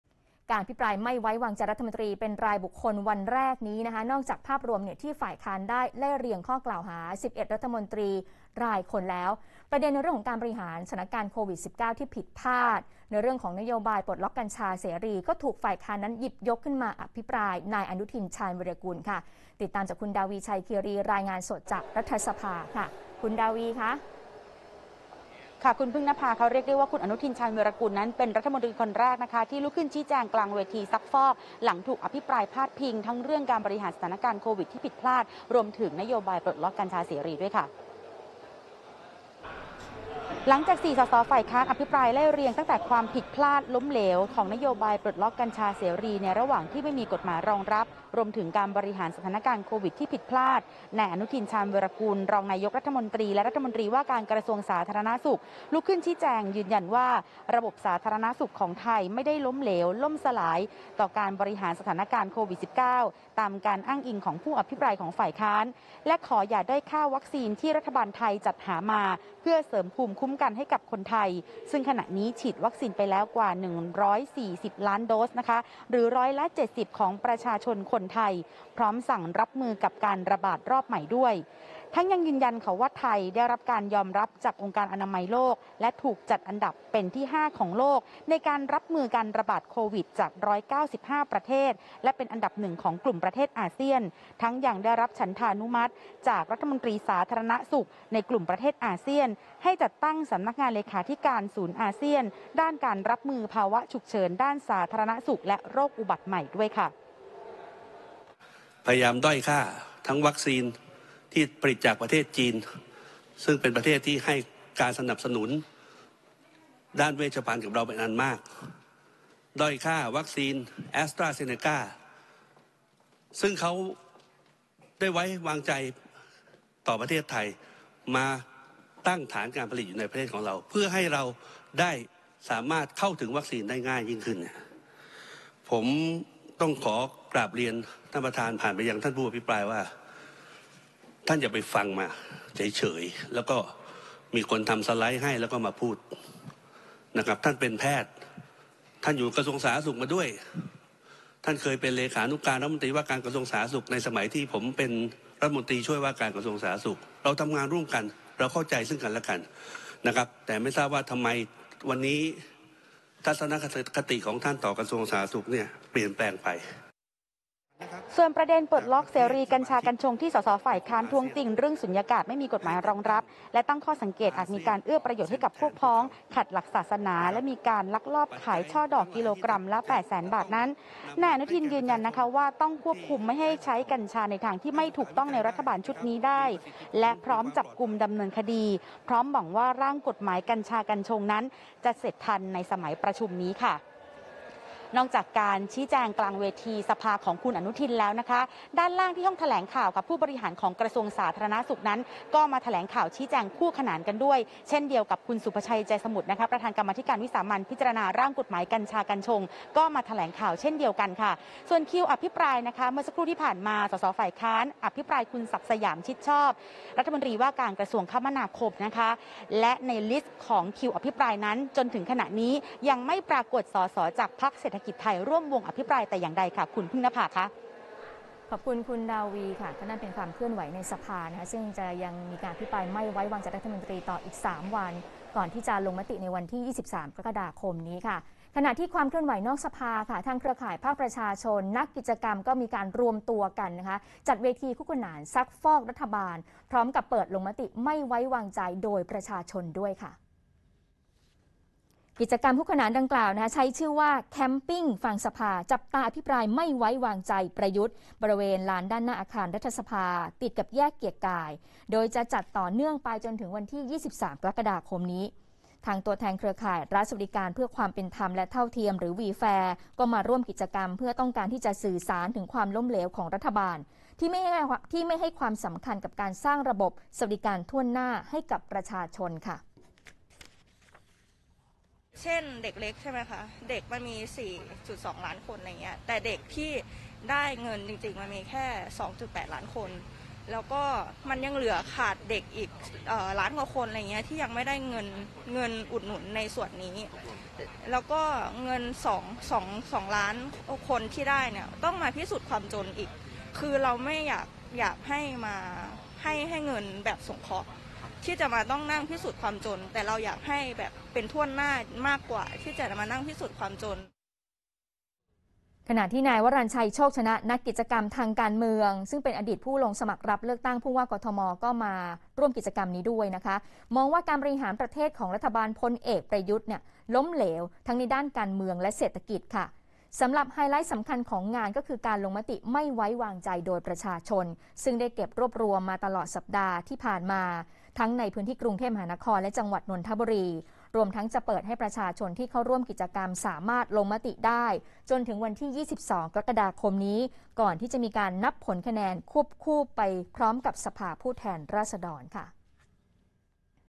"อนุทิน" แจงประเด็นแก้โควิด-ปลดล็อกกัญชา "ราษฎร" จัดอภิปรายนอกสภาฯ | ทันข่าว 16.00 น. | 19 ก.ค. 65
การอภิปรายไม่ไว้วางใจรัฐมนตรีเป็นรายบุคคลวันแรก (19 ก.ค. 65) นอกจากภาพรวมที่ฝ่ายค้านไล่เรียงข้อกล่าวหา 11 รัฐมนตรี รายคนแล้ว ประเด็นการบริหารสถานการณ์ โควิดผิดพลาดและนโยบายปลดล็อกกัญชาเสรี ถูกฝ่ายค้านหยิบยกมาอภิปรายนายอนุทิน ชาญวีรกูล ขณะที่นอกสภาฯ เครือข่ายประชาชน นักกิจกรรม รวมตัวกัน จัดเวทีคู่ขนานซักฟอกรัฐบาล พร้อมเปิดลงมติไม่ไว้วางใจโดยประชาชน ติดตามรายละเอียดจากคุณดาวี ไชยคีรี รายงานจากรัฐสภา